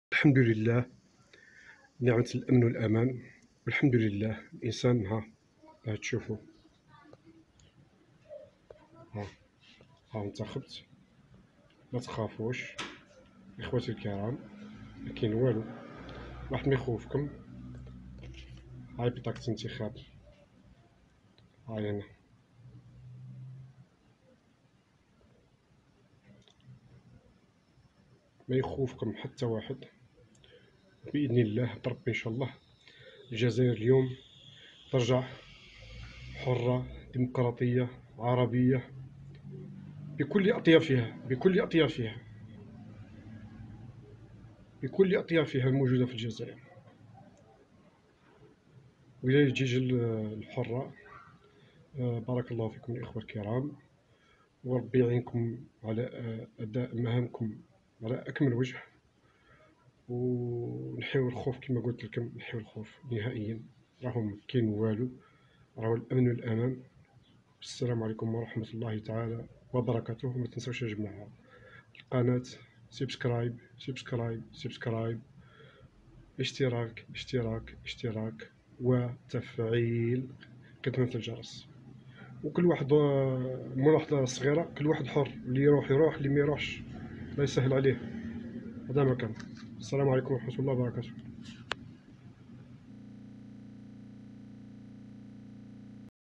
أدينا الواجب الوطني..وللخائفين روحوا بقلوبكم مطمئنين ولي يحب ييجي جيجل مرحبا به.
بفظل الله عز وجل  كانت  الاجواء رائعة كانت هناك  امطارا صباحا في مدينة  الميلية ،ثم في تمام التاسعة ونصف صحت  الاجواء  واطلت  الشمس في رابعة  النهار معلنة عن ميلاد فجر جديد لبلدنا ووطننا...الباقي يكمل من ينصب رئيسا  للجمهورية على مهامه كاملة كما وعدوا الشعب..وخاصة  الطبقة  الفقيرة و المعدمة  و المتوسطة .